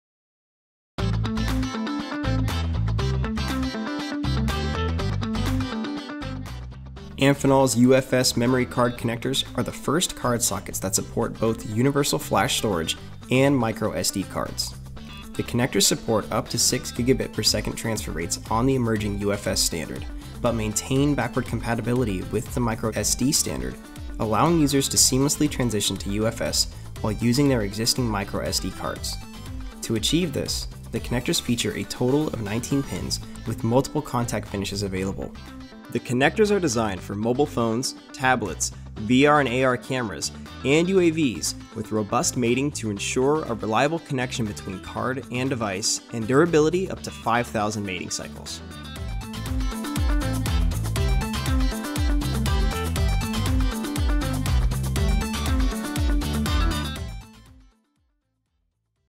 Amphenol UFS Memory Card Connectors | Digi-Key Daily
Amphenol’s UFS memory card connectors are the first card sockets that support both Universal Flash Storage and micro-SD cards.  The connectors support up to 6 Gbps transfer rates on the emerging UFS standard, but maintain backward compatibility with the micro-SD standard, allowing users to seamlessly transition to UFS while using their existing micro-SD cards.  To achieve this, the connectors feature a total of 19 pins, with multiple contact finishes available.  The connectors are designed for mobile phones, tablets, VR and AR cameras, and UAVs with robust mating to ensure a reliable connection between card and device and durability up to 10,000 mating cycles.